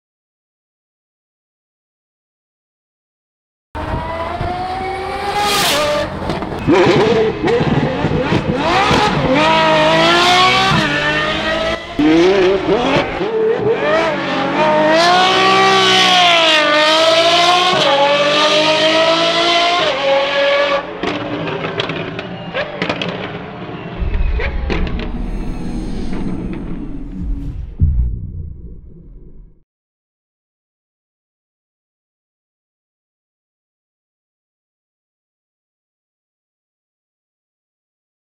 Apollo IE | Bahrain International Circuit (Raw Footage)
Enjoy the sounds of the Purple Dragon having some fun at Bahrain International Circuit during our recent